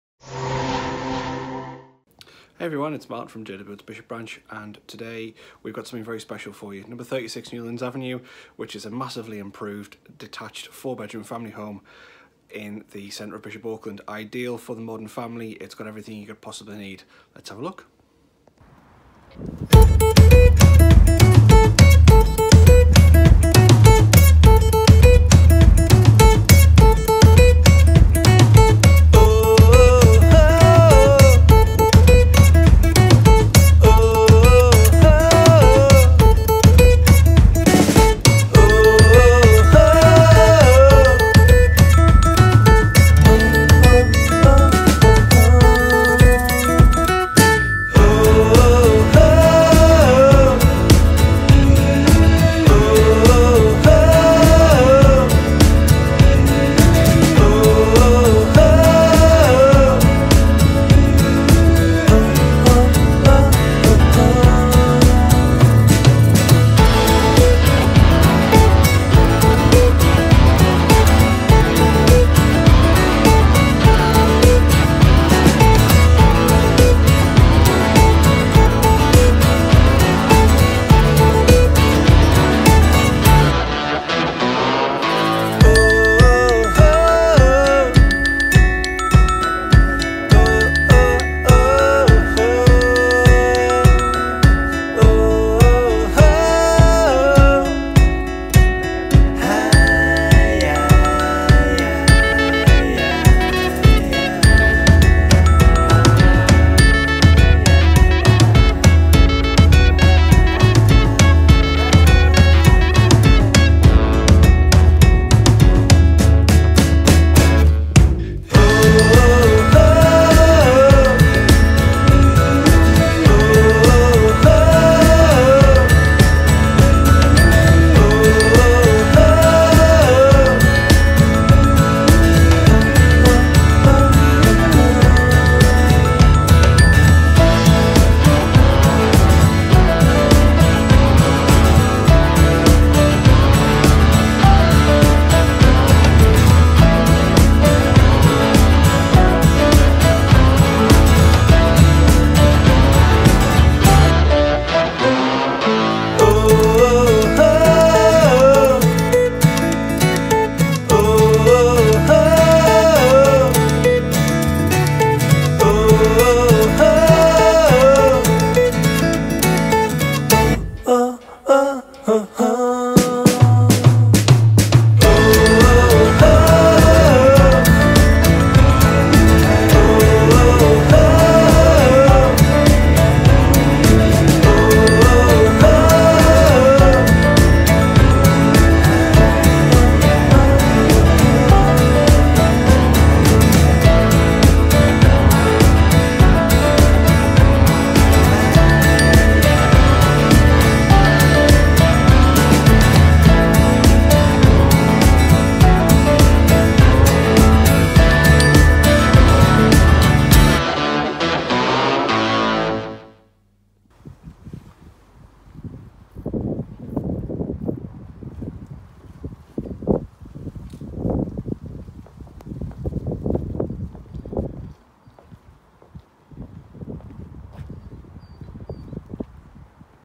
HOUSE FOR SALE. 36 Newlands Avenue, Bishop Auckland, Co. Durham, DL14 6AL.
Located in one of Bishop Auckland’s most sought after areas, this substantial family home is sure to be in high demand to a wide range of buyers. It has been improved throughout by our clients and now is offered to the market as a fabulous, modern home.

The spacious accommodation is traditionally laid out over two floors. It comprises of a large entrance hallway with feature herringbone style flooring,  living room with a study at the rear -  the perfect place to work from home as it has great views over the garden, a snug sitting room which could be used as a formal dining room, cloakroom / wc, and a truly spectacular family room at the rear.

This room is not only a great kitchen for any chef to enjoy but also space for the family to dine, and relax in. The kitchen units are in wood with white quartz worktops.

There is a utility room which houses the boiler and plumbing for the washing machine as well as more storage. Furthermore there is a ground floor shower room/wc.

On the first floor, there are four bedrooms arranged around a wide landing which has a handy reading spot with a large window providing ample light, and a family bathroom that is made up of a suite in white,  which again is exceptional.

Externally there are gardens to front, side and rear made up of lawns and artificial grass, there is also a shed which our clients use as a gym and a wooden Gazebo which is perfect for sitting outside, or housing a hot tub.

There is ample off-street parking on the block paved driveway, and a good sized garage which has a remote controlled roller shutter door.

This house is beautiful, and so we recommend viewing early to avoid missing out on this very rare opportunity for such a big, modern and comfortable home so close to the town centre, yet set in a quiet location.

Our clients have had plans passed for an extension to the front of the house which would create a master bedroom suite as well as more ground floor accommodation. Further details are available upon request.

This property sits on a great plot at the end of Newlands Avenue, which is one of the most desirable locations to live in Bishop Auckland town centre. As such it has all of the town's amenities on its door step.